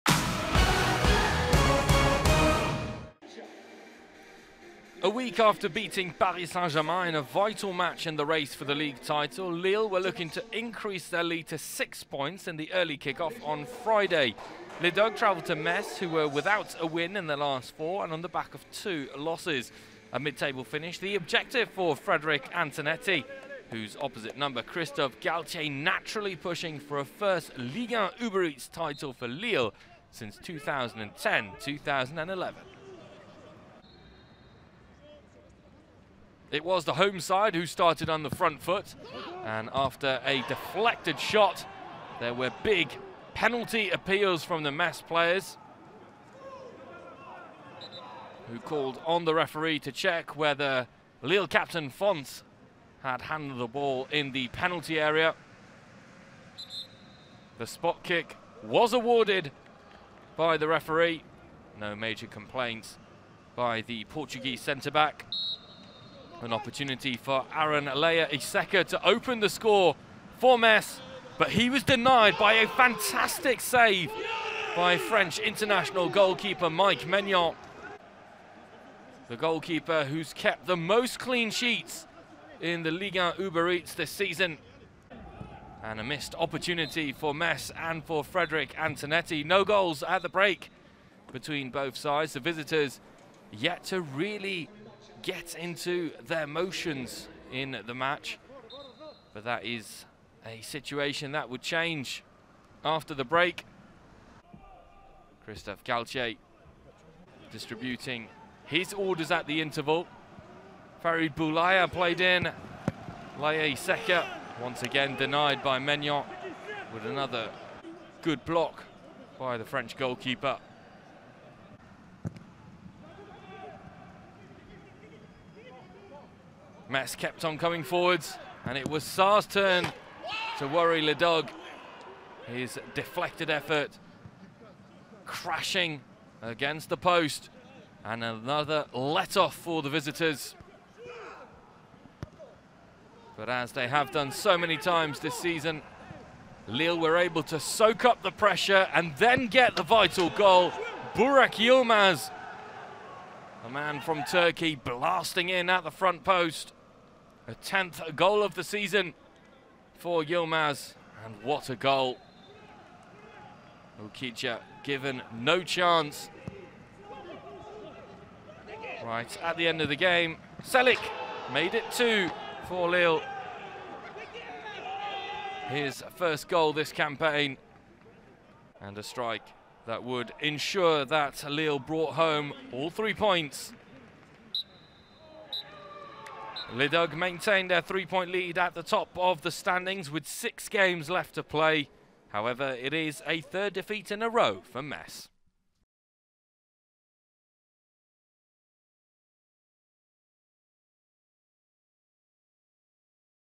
FC METZ - LOSC LILLE (0 - 2) - Highlights - (FCM - LOSC) / 2020-2021
FC METZ vs LOSC LILLE Highlights (0 - 2) in video.

Ligue 1 Uber Eats - Season 2020/2021 - Week 32 STADE MUNICIPAL SAINT SYMPHORIEN - Friday 9 April 2021

Goals : Burak YILMAZ (60' - LOSC LILLE) / Zeki CELIK (89' - LOSC LILLE)

FC METZ Team line-up : 16 - Alexandre OUKIDJA /21 - John BOYE (c) /3 - Matthieu UDOL /10 - Farid BOULAYA /18 - Fabien CENTONZE /19 - Habib MAIGA /24 - Aaron LEYA ISEKA /2 - Dylan BRONN /13 - Mamadou Lamine GUEYE /23 - Boubacar Dit Kiki KOUYATE /15 - Pape Matar SARR

LOSC LILLE Team line-up : 21 - Benjamin ANDRE /7 - Jonathan BAMBA /16 - Mike MAIGNAN /10 - Jonathan IKONE /24 - Boubakary SOUMARE /2 - Zeki CELIK /6 - José FONTE (c) /28 -  REINILDO /18 - Renato SANCHES /5 - Sven Adriaan BOTMAN /17 - Burak YILMAZ